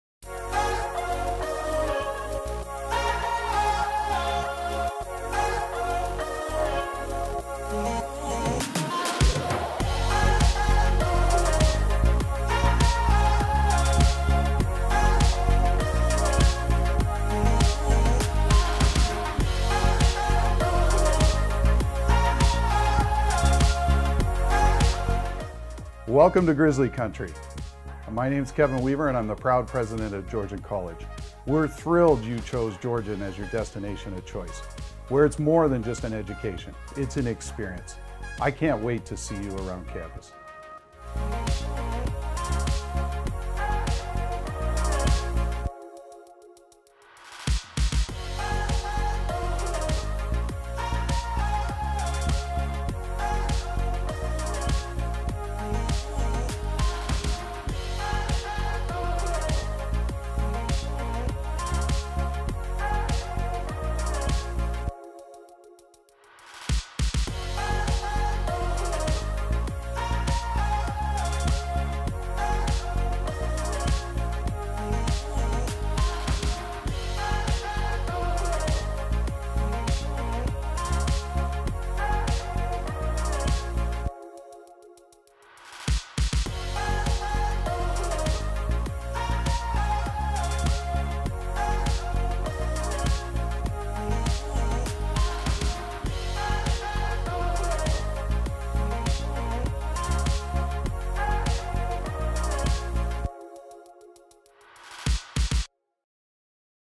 Welcome to your Midland Campus community
Welcome message for new Georgian College students including greetings from our President and CEO Kevin Weaver welcoming them to our community and images of local attractions.